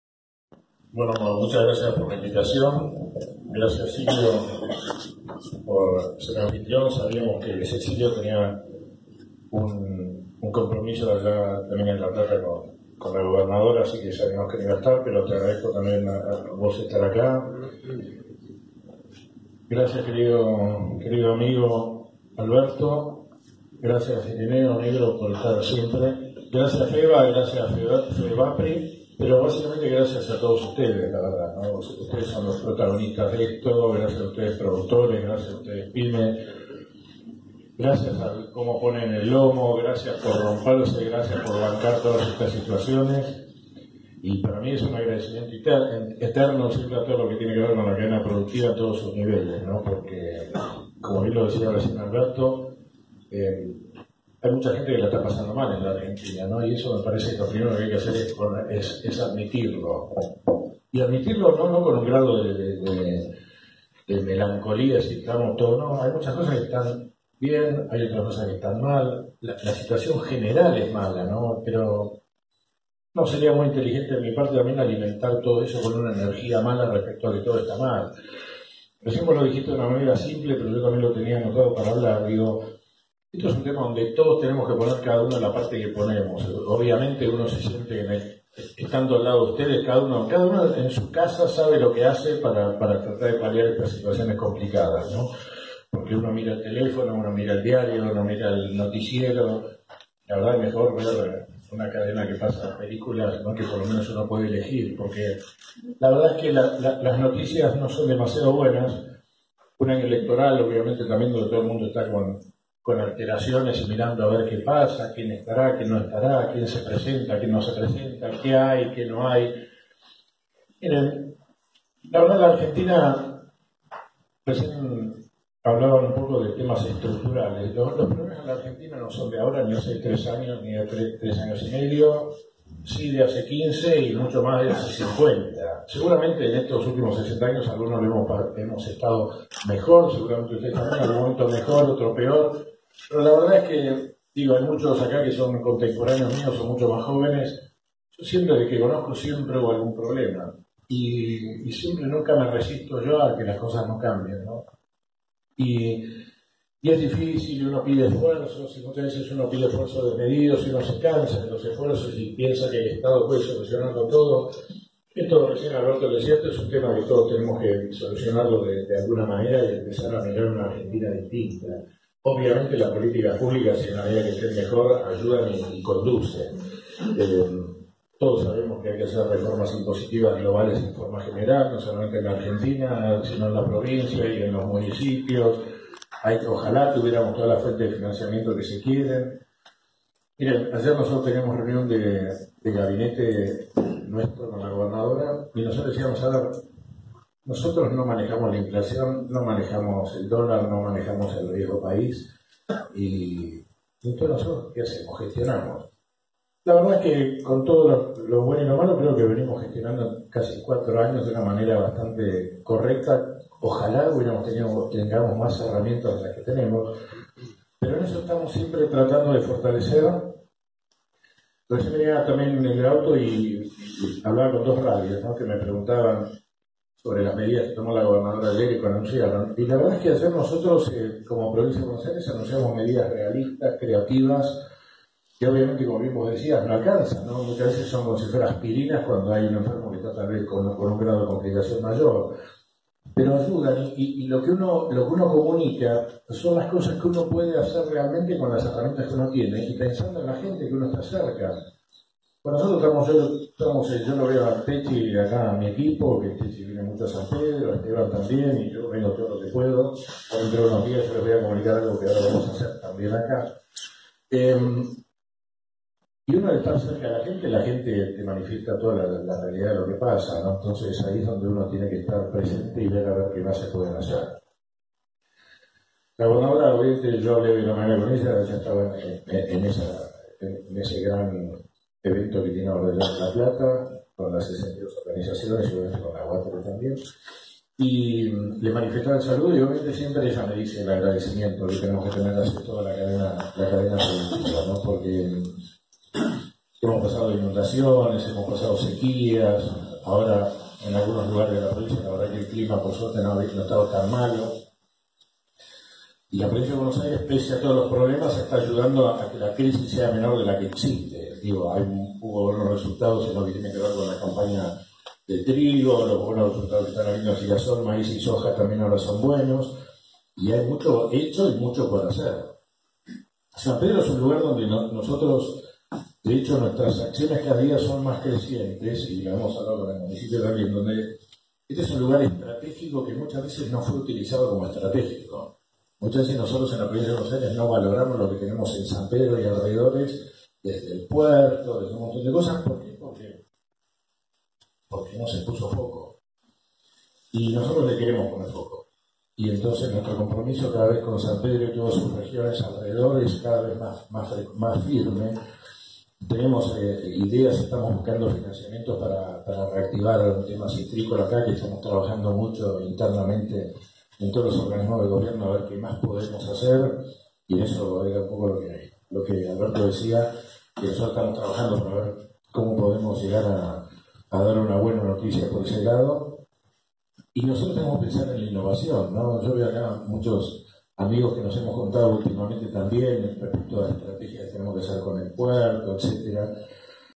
FEBAPRI - 1º Encuentro Regional de Productores Bonaerenses FEBA - FEBAPRI 2019
Leonardo Sarquís - Ministro de Agroindustria de la Provincia de Buenos Aires